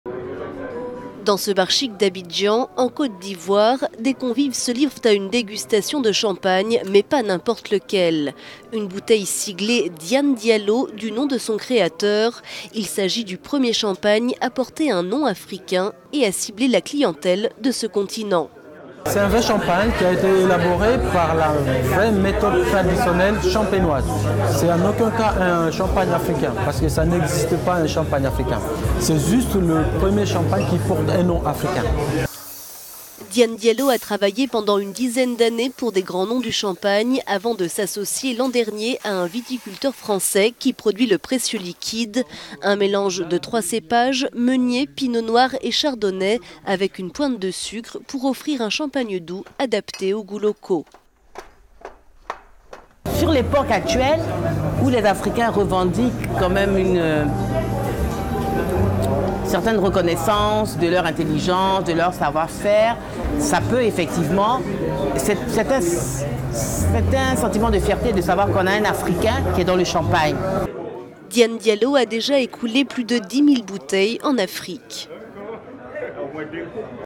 Un premier champagne au nom africain
Son créateur, Dian Diallo, s'est associé à un viticulteur champenois pour cibler la clientèle du continent africain.…